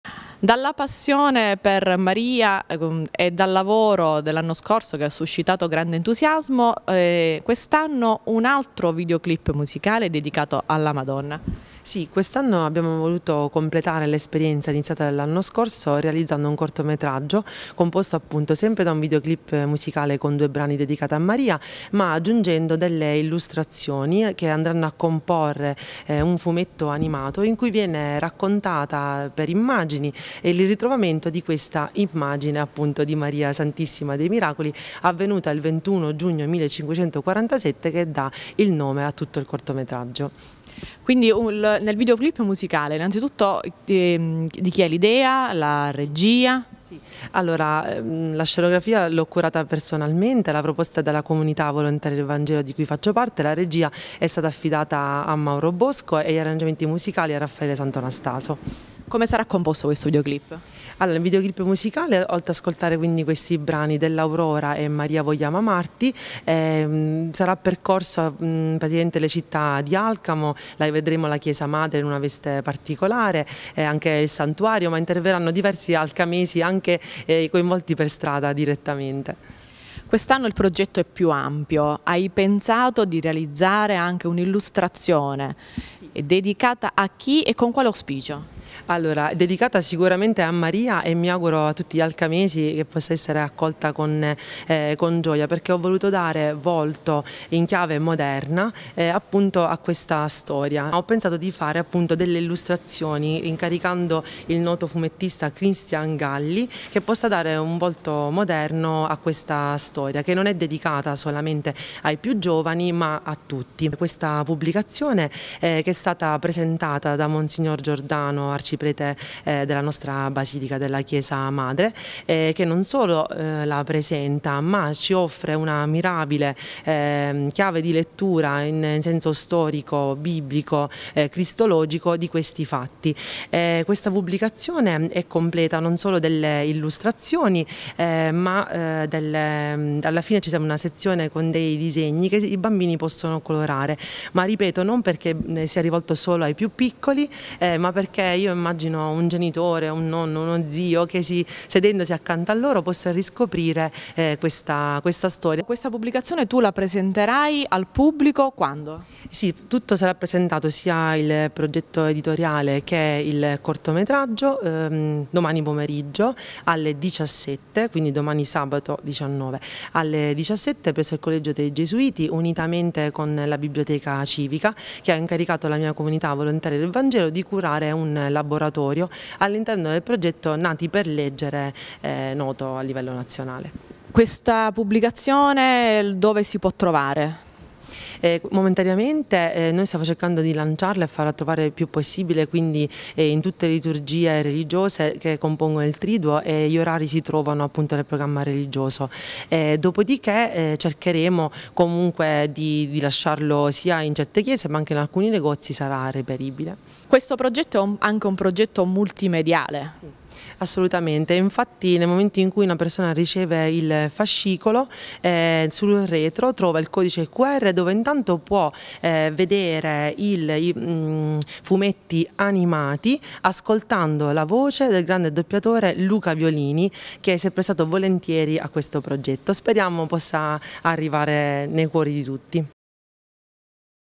Alcamo, nuovo video clip dedicato a Maria SS. dei Miracoli e storia a fumetti
Nuovo videoclip musicale dedicato a Maria SS dei Miracoli. Realizzato dalla comunità volontarie del vangelo. Quest’anno il programma prevede anche un’illustrazione grafica